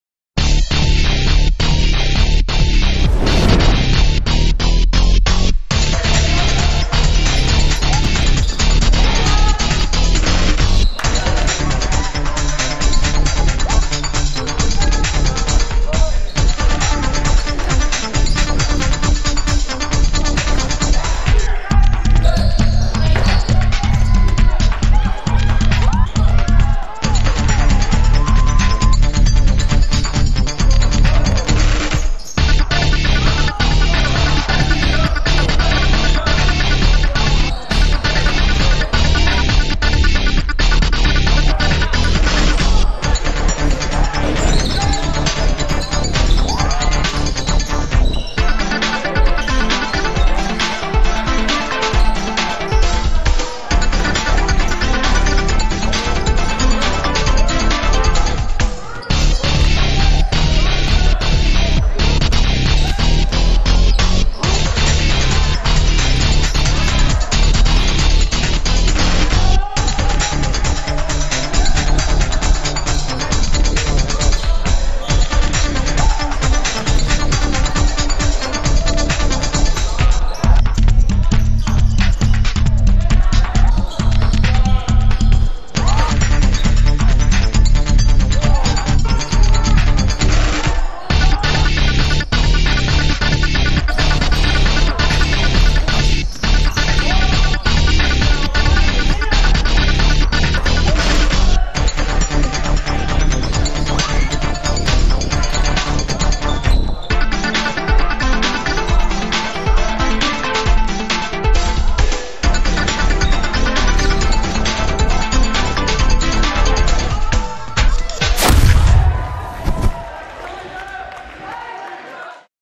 JNB Year in Review - Shots
"Shots" section from the JN Burnett Junior Basketball 2010-11 Year in Review video.